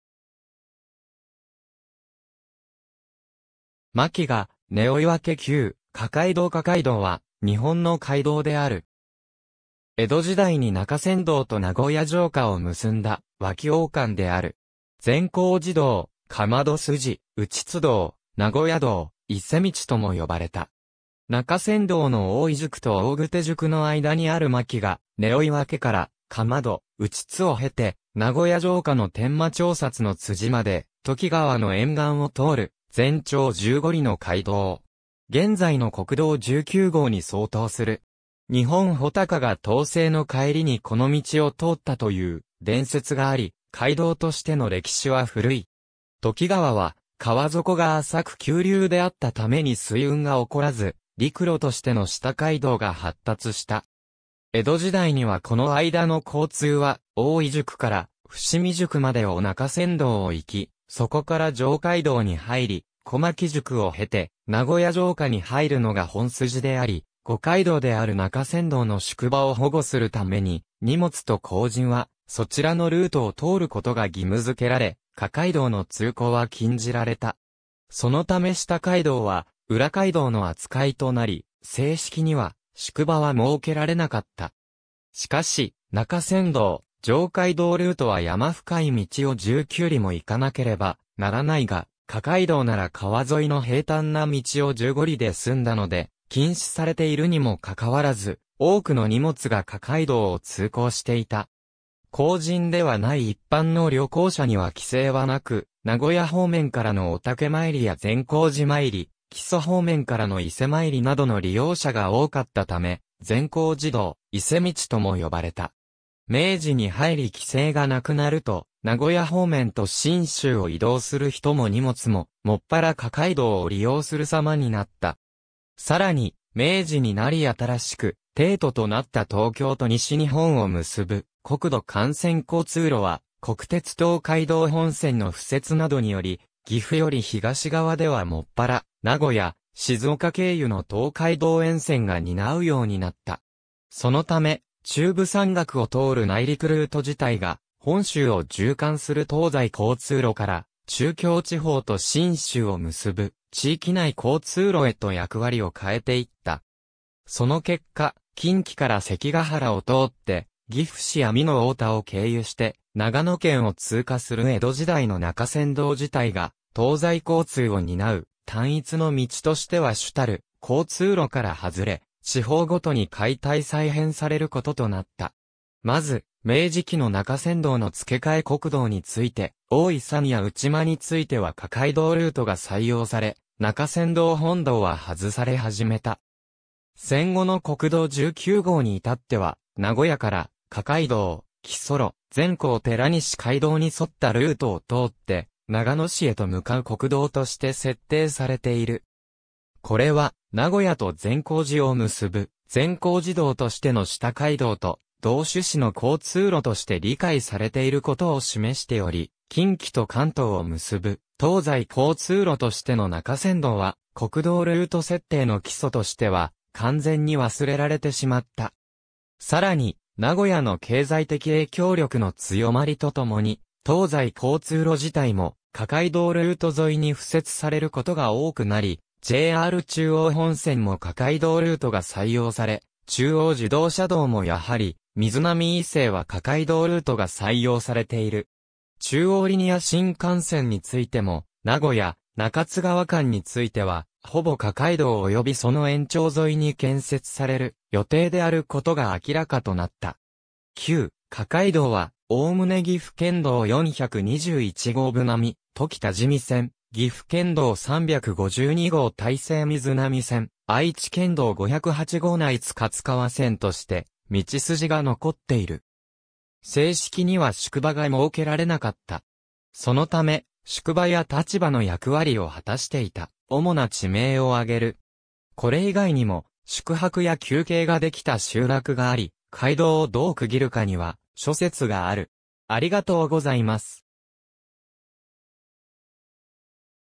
下街道 (善光寺道)
槙ヶ根追分 旧・下街道（愛知県道508号内津勝川線、撮影場所：愛知県春日井市柏井町） 下街道（したかいどう）は、日本の街道である。
江戸時代に中山道と名古屋城下を結んだ脇往還である。
善光寺道、釜戸筋、内津道、名古屋道、伊勢道とも呼ばれた。
中山道の大井宿と大湫宿の間にある槙ヶ根追分から、釜戸、内津を経て名古屋城下の伝馬町札の辻まで、土岐川（庄内川）の沿岸を通る全長15里の街道。
現在の国道19号に相当する。
「日本武尊（やまとたけるのみこと）が東征の帰りにこの道を通った」と言う伝説があり、街道としての歴史は古い。
土岐川は、川底が浅く急流であったために水運が起こらず、陸路としての下街道が発達した。
江戸時代にはこの間の交通は、大井宿から伏見宿までを中山道を行き、そこから上街道に入り、小牧宿を経て名古屋城下に入るのが本筋であり、五街道である中山道の宿場を保護するために荷物と公人はそちらのルートを通る事が義務づけられ、下街道の通行は禁じられた。
そのため下街道は裏街道の扱いとなり、正式には宿場は設けられなかった。
しかし、中山道・上街道ルートは山深い道を19里も行かなければならないが、下街道なら川沿いの平たんな道を15里で済んだので、禁止されているにもかかわらず、多くの荷物が下街道を通行していた（上街道と中山道の宿場は、たびたび下街道の商人荷物輸送の停止を訴え出ているが、これが逆に下街道の利用が多かったことを示している）。
公人ではない一般の旅行者には規制はなく、名古屋方面からの御岳参りや善光寺参り、木曽方面からの伊勢参りなどの利用者が多かったため、善光寺道、伊勢道とも呼ばれた。
明治に入り規制がなくなると、名古屋方面と（伊那谷以外の）信州を移動する人も荷物も、専ら下街道を利用する様になった。
さらに、明治になり新しく帝都となった東京と西日本を結ぶ国土幹線交通路は、国鉄東海道本線の敷設などにより、岐阜より東側ではもっぱら名古屋・静岡経由の東海道沿線が担うようになった。
そのため、中部山岳を通る内陸ルート自体が、本州を縦貫する東西交通路から、中京地方と信州を結ぶ地域内交通路へと役割を変えていった。
その結果、近畿から関が原を通って、（名古屋へ南下迂回せず）岐阜市や美濃太田を経由して長野県を通過する江戸時代の中山道自体が、東西交通を担う単一の道としては主たる交通路から外れ、地方ごとに解体再編されることとなった。
まず、明治期の中山道の付け替え国道について、大井-山野内（瑞浪市）間については下街道ルートが採用され中山道本道は外され始めた。
戦後の国道19号に至っては、名古屋から、下街道・木曽路・善光寺西街道に沿ったルートを通って長野市へと向かう国道として設定されている。
これは名古屋と善光寺（長野市）を結ぶ、「善光寺道としての下街道」と同趣旨の交通路として理解されていることを示しており、「近畿と関東を結ぶ東西交通路としての中山道」は、国道ルート設定の基礎としては完全に忘れられてしまった。
さらに、名古屋の経済的影響力の強まりとともに、東西交通路自体も、下街道ルート沿いに敷設されることが多くなり、JR中央本線も下街道ルートが採用され、中央自動車道もやはり、瑞浪以西は下街道ルートが採用されている。
中央リニア新幹線についても、名古屋・中津川間については、ほぼ下街道およびその延長沿いに建設される予定であることが明らかとなった。
旧・下街道は、おおむね岐阜県道421号武並土岐多治見線・岐阜県道352号大西瑞浪線・愛知県道508号内津勝川線として道筋が残っている。
正式には宿場が設けられなかった。
そのため、宿場や立場の役割を果たしていた主な地名を挙げる。
これ以外にも宿泊や休憩ができた集落があり、街道をどう区切るかには諸説がある。